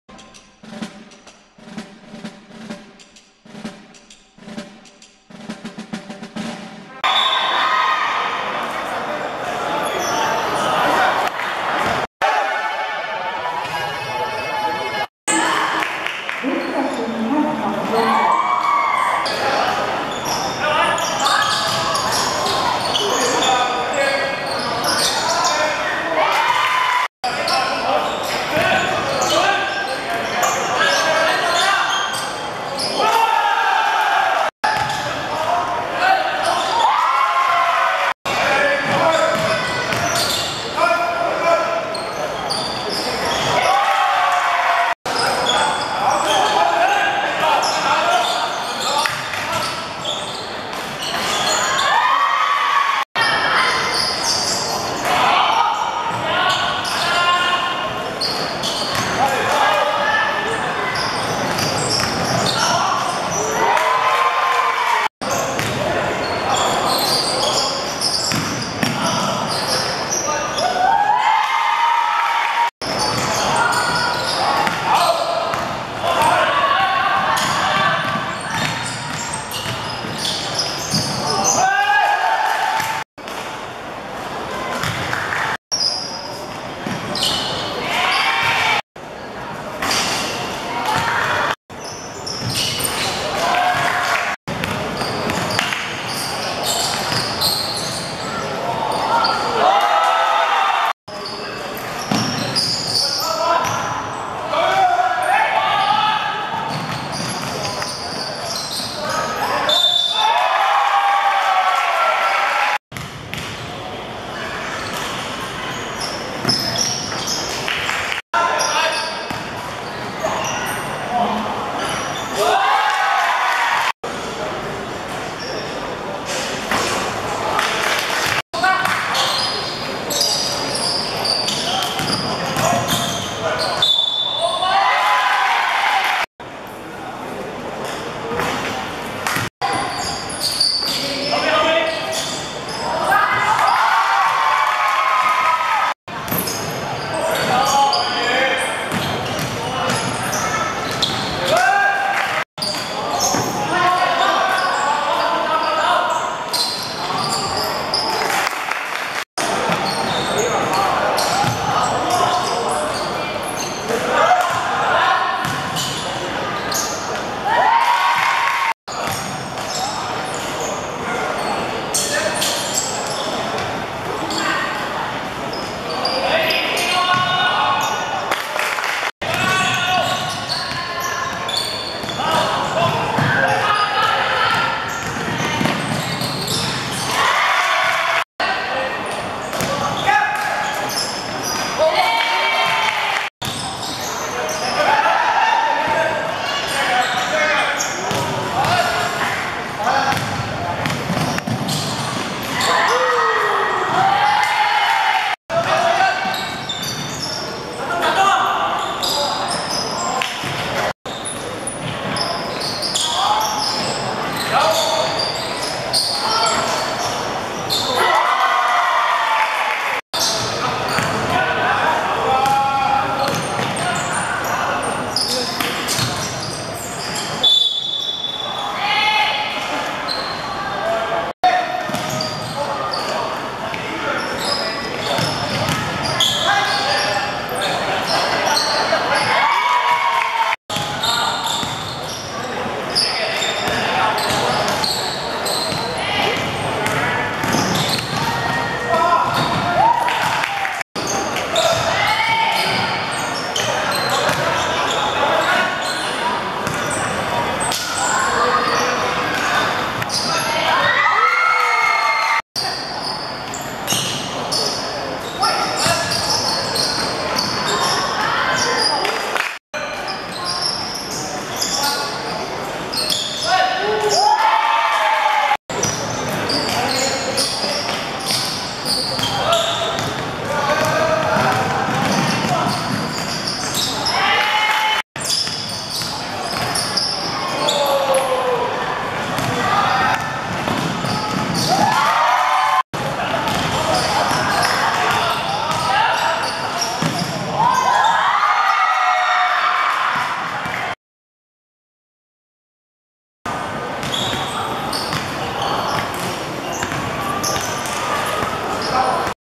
天水圍循道vs培聖(2013.9.27.元朗學界籃球甲組)精華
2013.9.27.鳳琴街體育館
天水圍循道(黃) 52 : 17 培聖(藍)